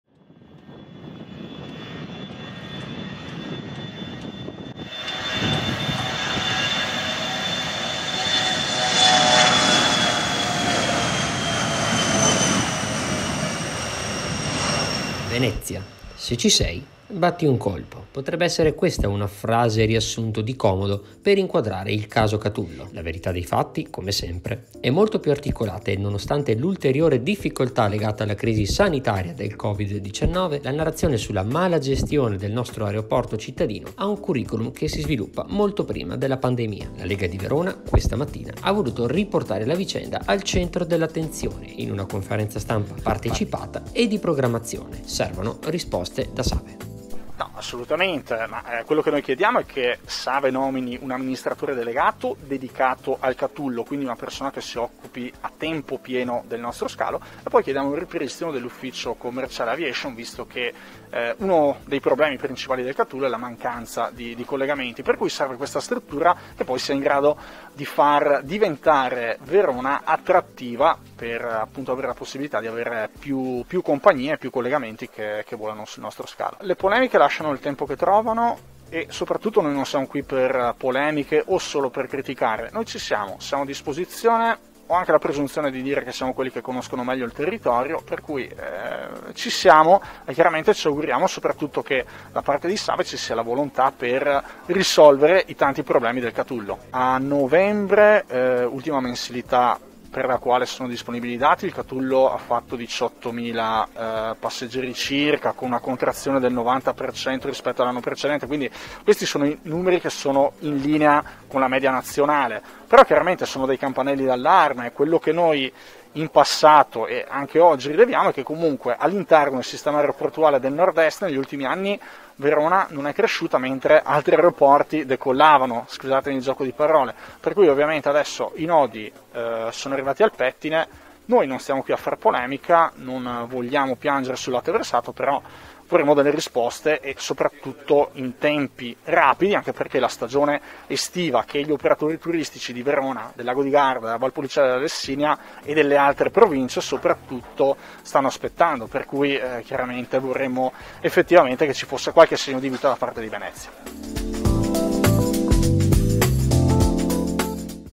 Aeroporto Catullo, pressing della Lega: "SAVE, se c'è, batta un colpo, portiamo 'il caso' in Comune"
"Venezia: se ci sei, batti un colpo".

Potrebbe essere questa una frase-riassunto di comodo per inquadrare il "caso Catullo"; la verità dei fatti, come sempre, è molto più articolata e nonostante l'ulteriore difficoltà legata alla crisi sanitaria del COVID19, la narrazione sulla mala gestione del nostro Aeroporto cittadino ha un curriculum che si sviluppa molto prima della pandemia.

La Lega di Verona, questa mattina, ha voluto riportare la vicenda al centro di una conferenza stampa partecipata e di "programmazione": servono risposte da SAVE. La nostra intervista all'europarlamentare Paolo Borchia.